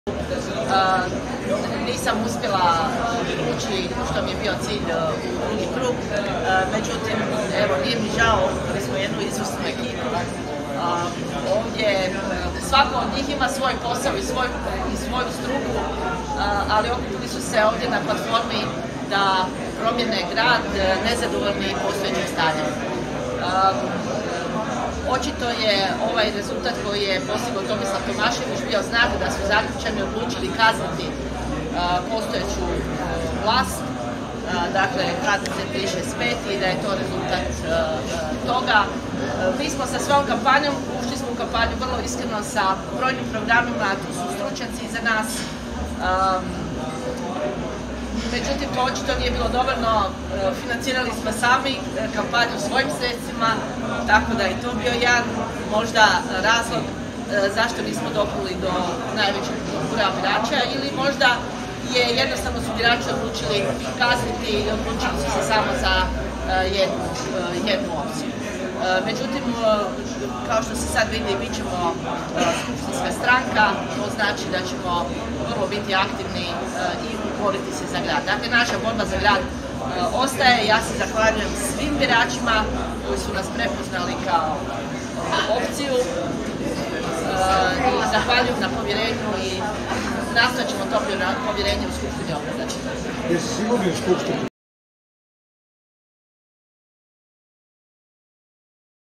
Vesna Škare Ožbolt komentirala je prve službene rezultate DIP-a
VIDEO: Zoe Šarlija/PIXSELL
Photo gallery: